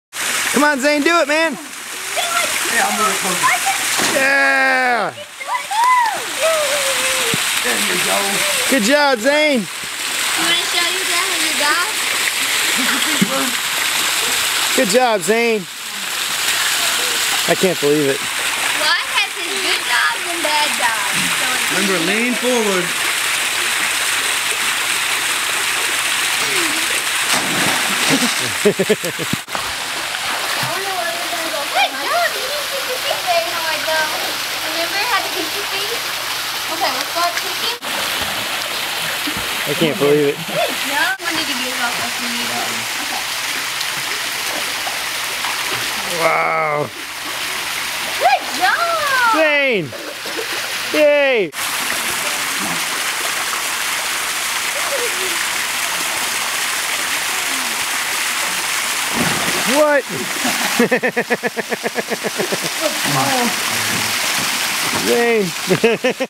Swim Lessons
Wyatt and Zane both took swim lessons this week and it has paid off immensely, especially w/Zane as he's gotten over his fear of water and is loving it! Thank you Alissa & Bruce for being such rocking teachers!